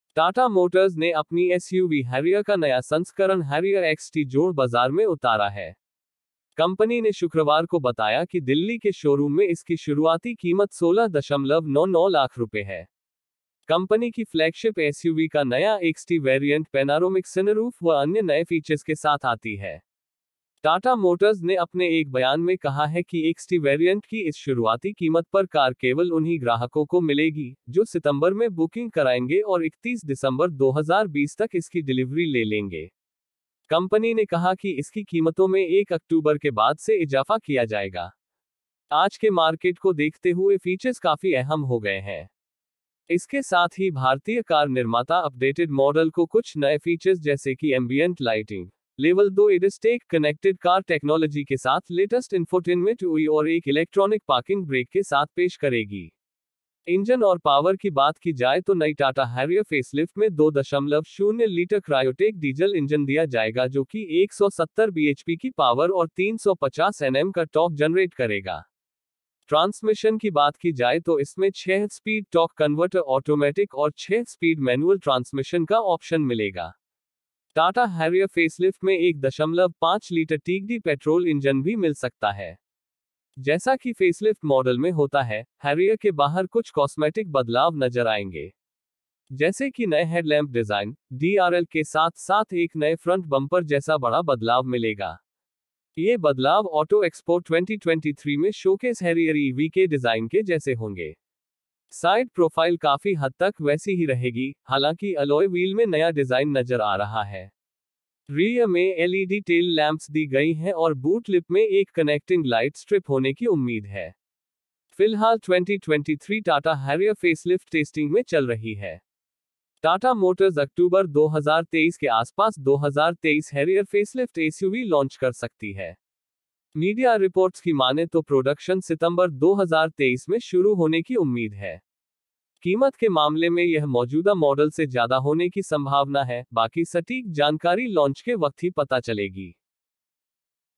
Tata Motors ने लॉन्‍च किया सनरूप के साथ Harrier का सस्‍ता वर्जन, 1 अक्‍टूबर से बढ़ जाएगी कीमत...
covered topics